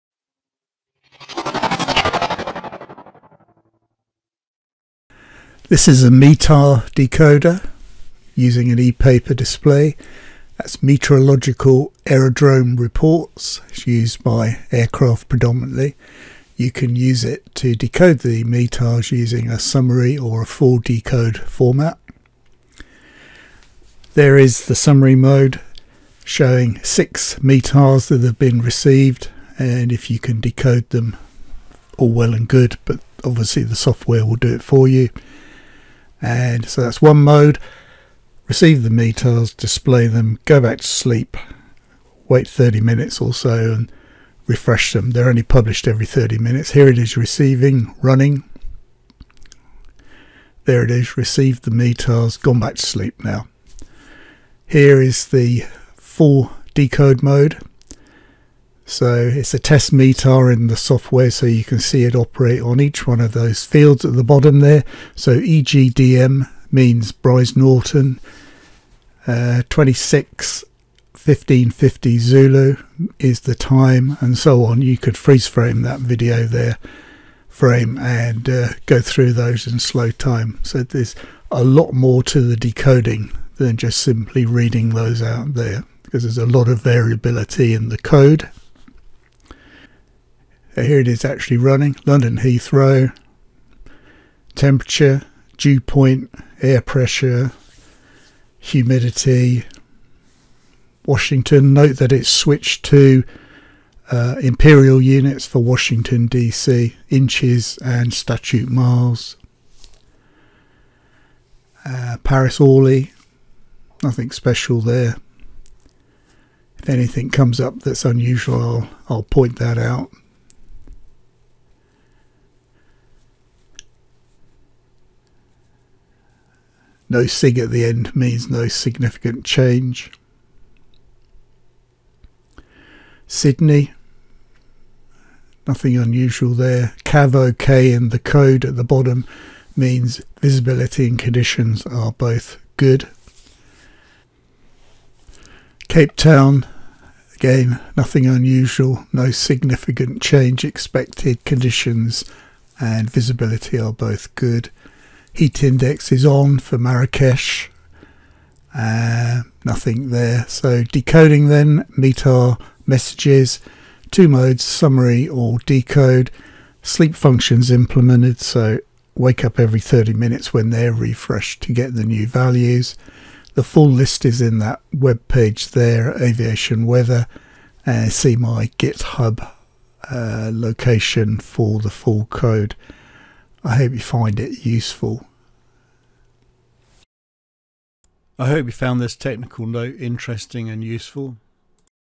Tech Note 153 - ESP32 METAR (METeorological Aerodrome Reports) Decoder
Meteorological Aerodrome Reports (METARs) are available worldwide and there's a station near you. They looks like this:
EGLL 131820Z AUTO 26006KT 9999 NCD 10/03 Q1031 NOSIG
But contain a lot of weather information